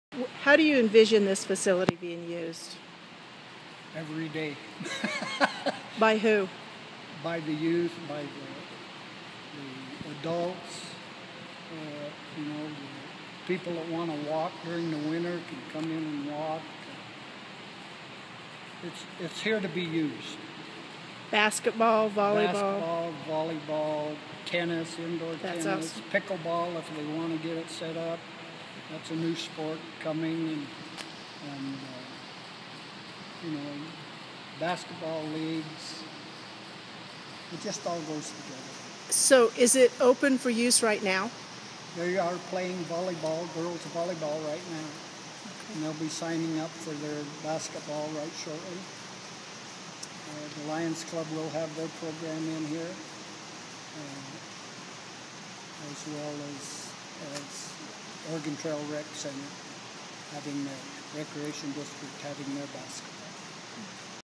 Oregon Trail Recreation Center
A new community recreation center was dedicated Oct. 7 in Burley. By Laurie Welch Times-News